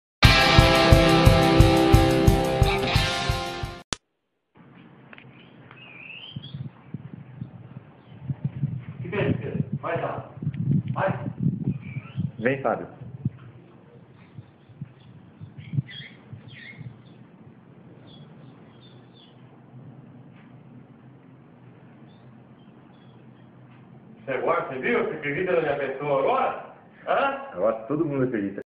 COLEIRO GALANDO FÊMEA DE TRINCA RARIDADE
coleiro novo , mas mostra-se bom galador , não perdoou nem a fêmea de trinca.

vídeo compartilhado via whatssap na região de Itajaí sc